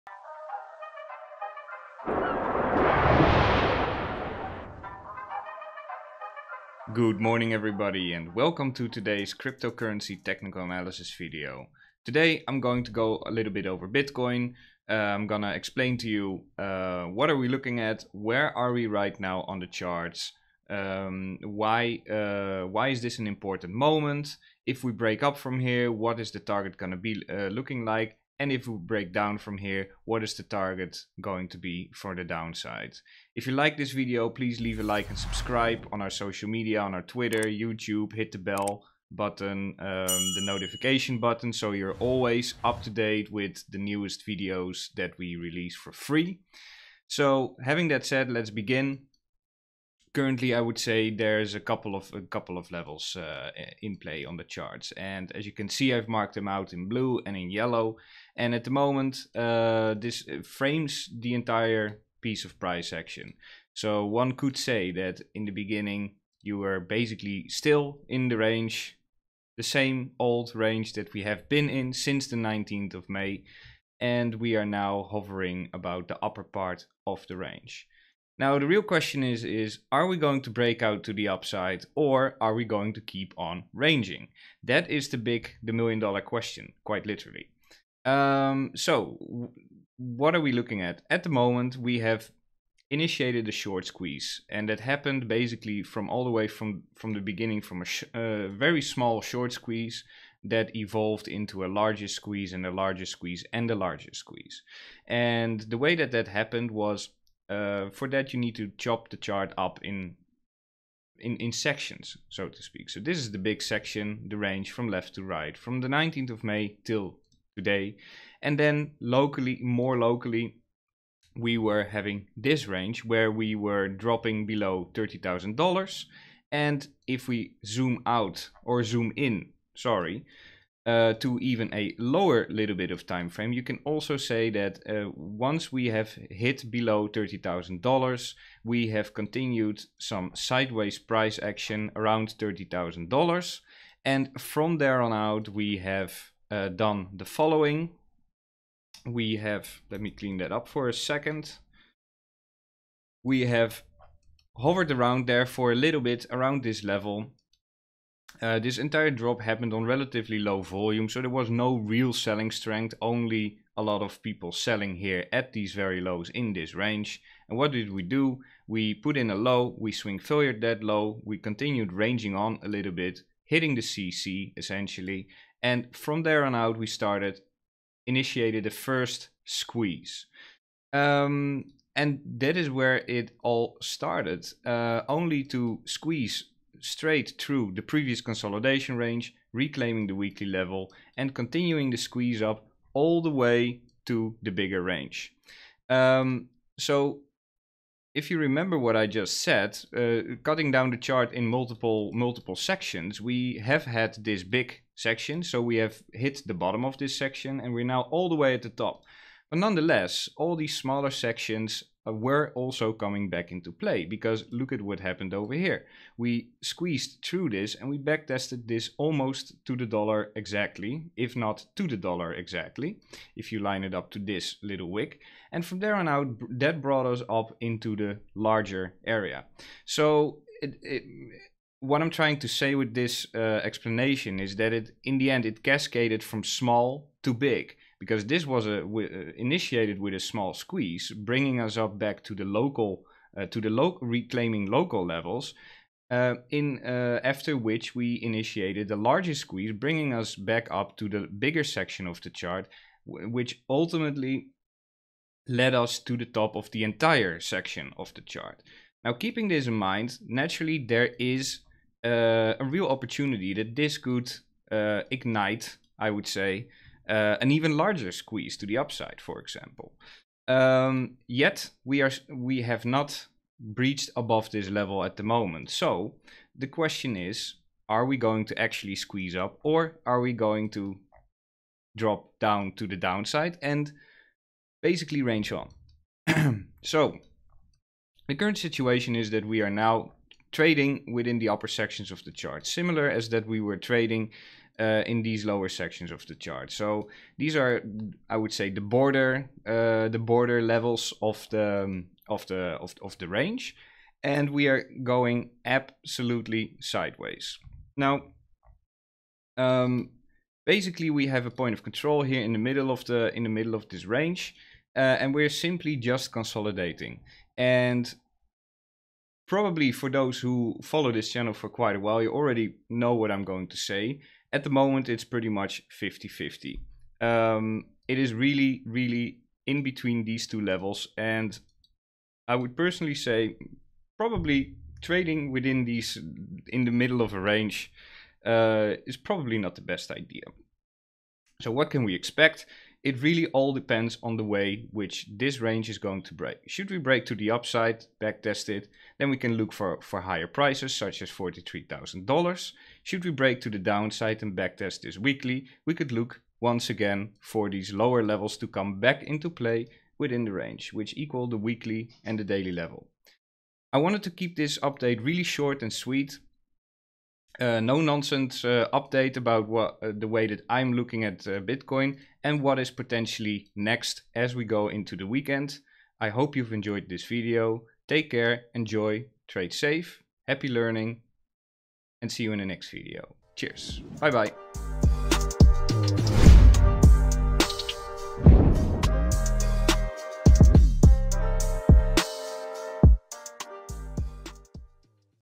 💰😨😱 BITCOIN PUMP AND DUMP?!! - What is next?
BTC Bitcoin Technical Analysis live now price prediction today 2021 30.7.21 $BTC has squeezed upwards and is now reaching important levels. Will the bulls be able to take over the bears and push for higher prices? Or will the bears prevail once again?